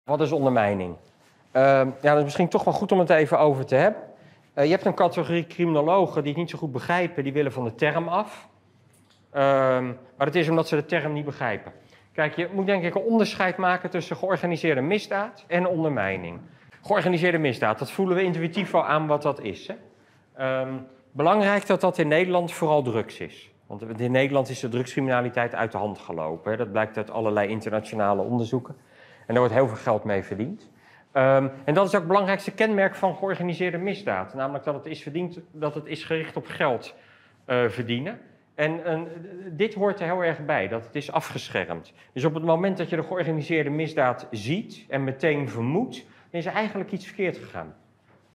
Edward vd Torre - toelichting terminologie (2)
Tijdens het symposium 'Veilige Vakantieparken' op 6 december in Zwolle, vertelde bestuurskundige en criminoloog Edward van der Torre over de termen georganiseerde misdaad en ondermijnende criminaliteit